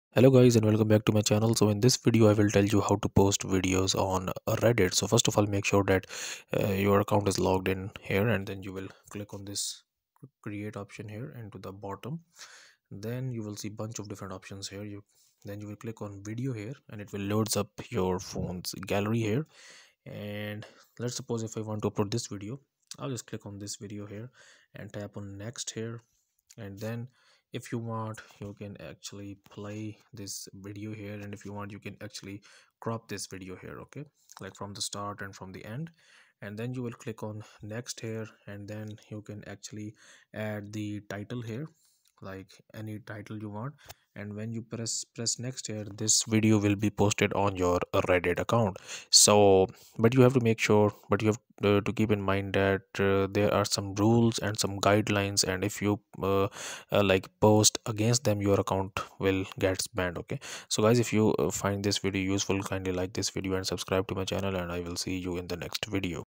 How to Post Videos on Reddit App (2023)
Welcome to our tutorial on how to post videos on the Reddit app in 2023. If you're looking to share a video with the Reddit community, we've got you covered. In this video, we'll show you how to upload a video to Reddit on both iOS and Android devices. Our step-by-step instructions will guide you through the process, so you can easily share your video content. We'll also provide some tips and tricks for maximizing engagement with your video posts. So, if you're wondering how to post videos on the Reddit app, stick around! We've got you covered. Follow along with our easy guide and you'll be sharing your videos with the world in no time.